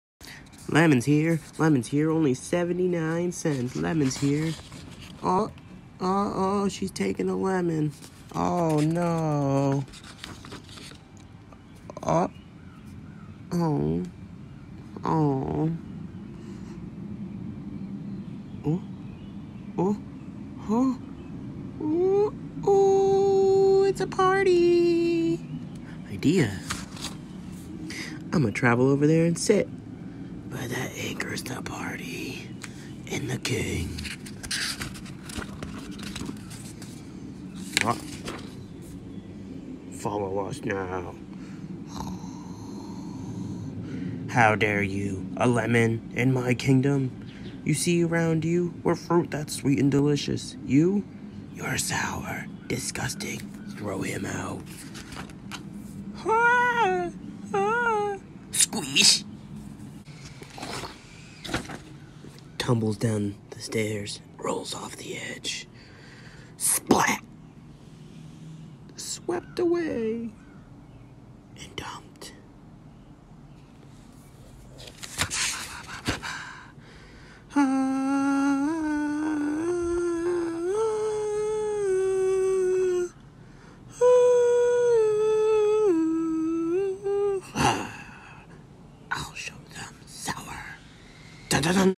THE LAST LEMON PT1
THE LAST LEMON PART 1

Follow the journey of one grocery store's last Lemon after being thrown away by King Melon.